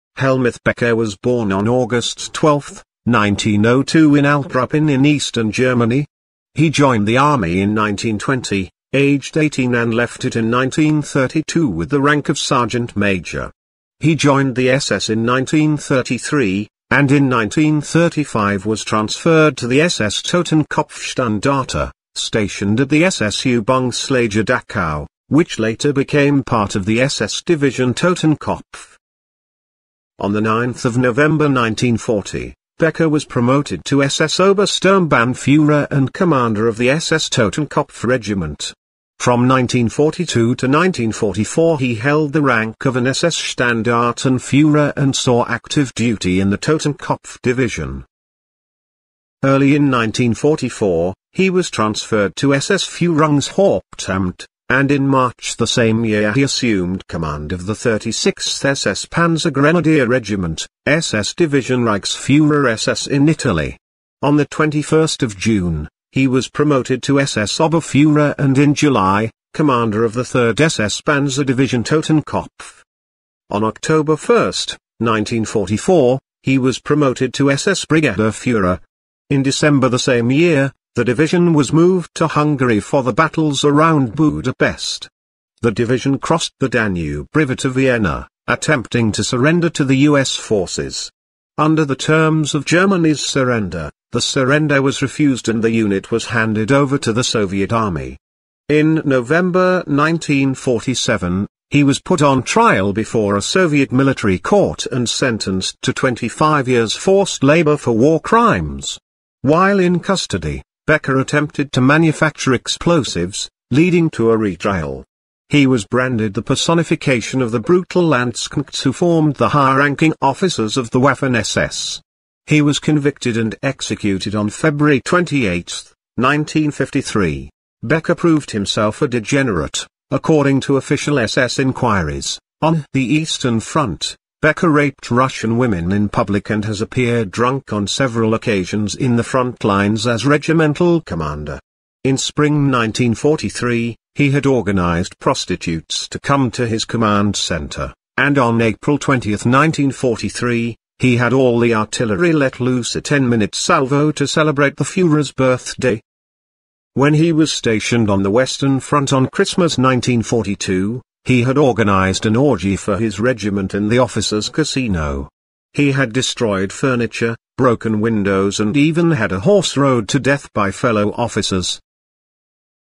Hellmuth Becker Biography
Hellmuth Becker (12 August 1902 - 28 February 1953) was a German SS commander during the Nazi era. In World War II, he led the SS Division Totenkopf and was a recipient of the Knight's Cross of the Iron Cross with Oak Leaves. Post-war, Becker was tried by Soviet authorities twice, for war crimes and sabotage and was executed in 1953, after the second trial.

Google Images